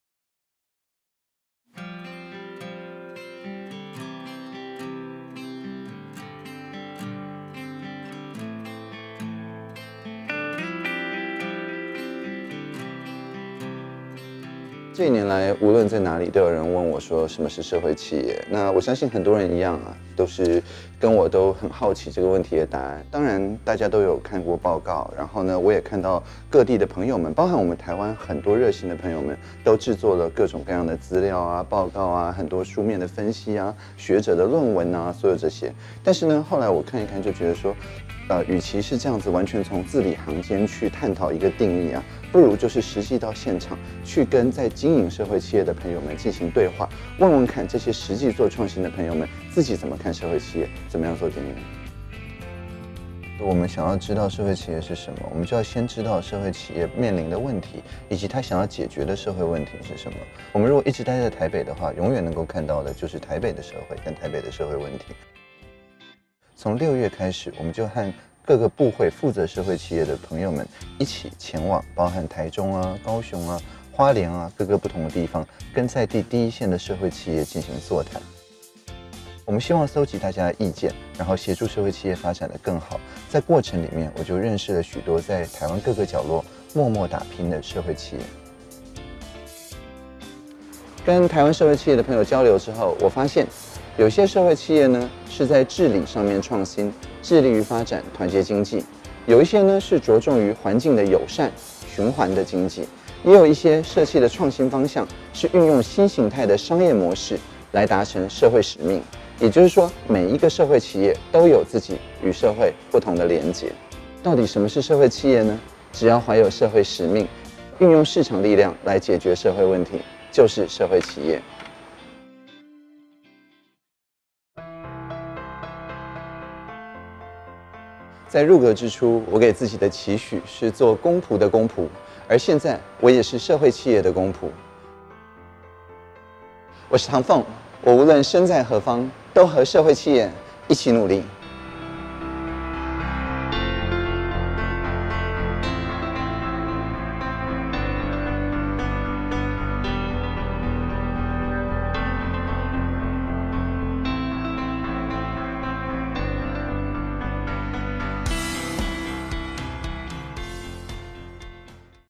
社會企業大哉問 What is "Social Enterprise"?
許多人曾經問我，「什麼是社會企業？」

我想，這個問題的答案，就在每個社會企業的故事裡。
社會創新的模式各自不同，但有著共同的目標，都是為了解決社會問題。

這一年來，我陸續與台灣各地的社會企業交流，充分感受到他們的能量和影響力。

我認為，政府在加強各界對社會企業的認識之外，更應該創造良好的環境，讓社會企業可以得到更充足的資源和協助。

在許多同仁的努力下，位於空總基地的「社會創新實驗中心」將在本周三正式開幕。這是一個「開放、群聚、試驗」的空間，讓所有的社會企業能夠相互合作、實現共好。

未來每周三，上午 10 點到下午 2 點，我也會固定在社會創新實驗中心辦公，讓各界朋友可以直接提出問題和建議，一起參與社會企業政策制定的過程。

10/18 開始，歡迎大家來社會創新實驗中心，一起支持社會企業。